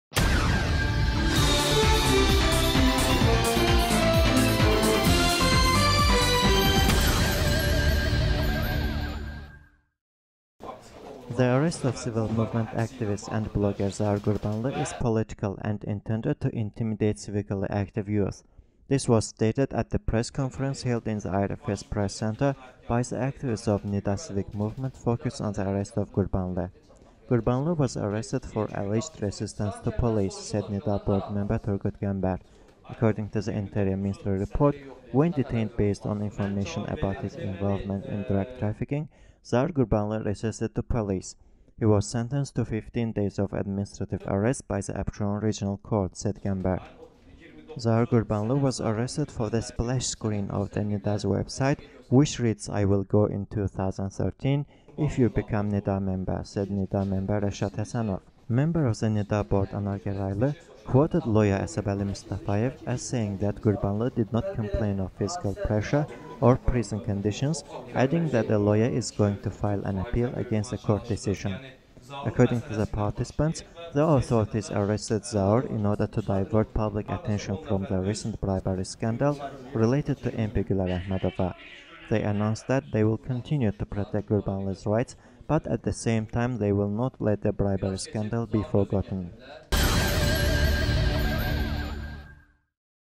ARREST OF ZAUR GURBANLI IS TO INTIMIDATE YOUNG PEOPLE
The arrest of civil movement activist and blogger Zaur Gurbanli is political and intended to intimidate civically active youth.  This was stated at the press conference held in the IRFS press center by the activists of NIDA civic movement, focused on the arrest of Gurbanli.
Gurbanli was arrested for alleged resistance to police, said NIDA board member Turgut Gambar.

"According to the Interior Ministry report, when detained based on information about his involvement in drug trafficking Zaur Gurbanli resisted to police. He was sentenced to 15 days of administrative arrest by the Absheron Regional Court" said Gambar.

"Zaur Gurbanli was arrested for the splash screen of the NIDA's website, which reads 'I will go in 2013, if you become NIDA member' " said NIDA member Reshad Hesenov.
Member of the NİDA Board, Anar Gerayli, quoted lawyer Asabali Mustafayev as saying that Gurbanli did not complain of physical pressure or prison conditions, adding that the lawyer is going to file an appeal against the court decision.

According to the participants, the authorities arrested Zaur in order to divert public attention from the recent bribery scandal related to MP Guler Ahmedova. They announced that they'll continue to protect Gurbanli's rights, but at the same time will not let the bribery scandal be forgotten.